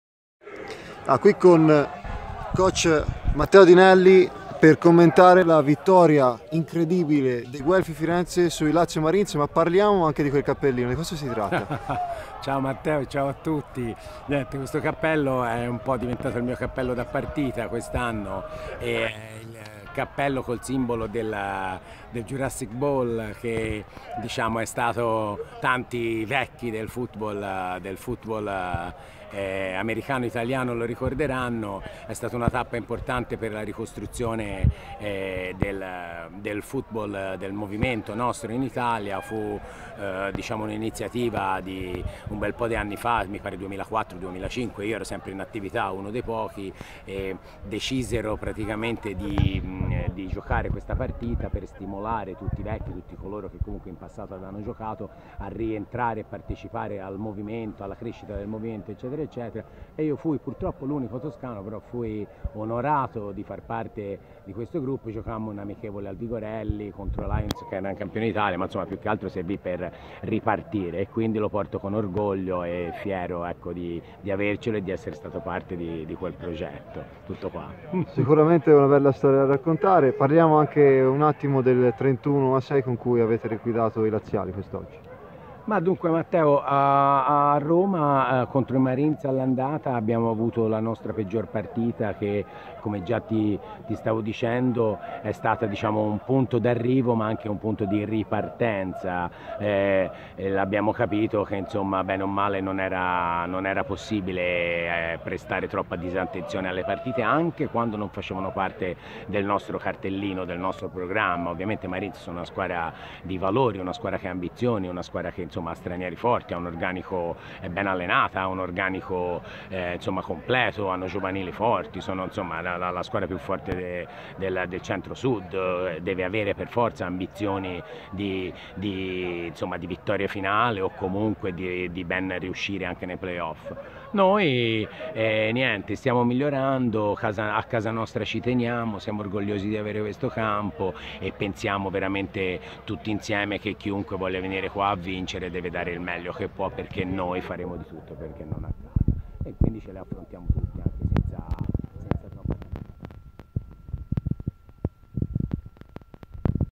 Guelfi Vs Marines, la parola a coach Dinelli
Le impressioni a caldo dell'head coach dei viola dopo la strabiliante vittoria per 31-6 contro i Lazio Marines.